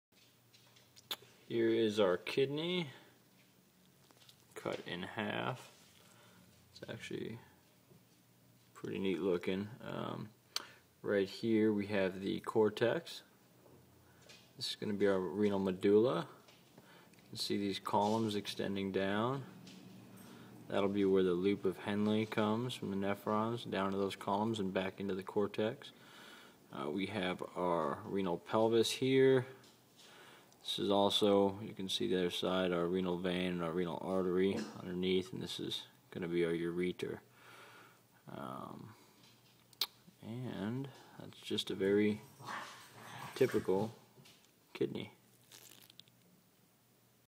Colorful Kidney Dissected.
In this video we examine the dissected kidney and identify the renal cortex, medulla, pelvis, columns, and discuss briefly the placement of nephrons and filtration.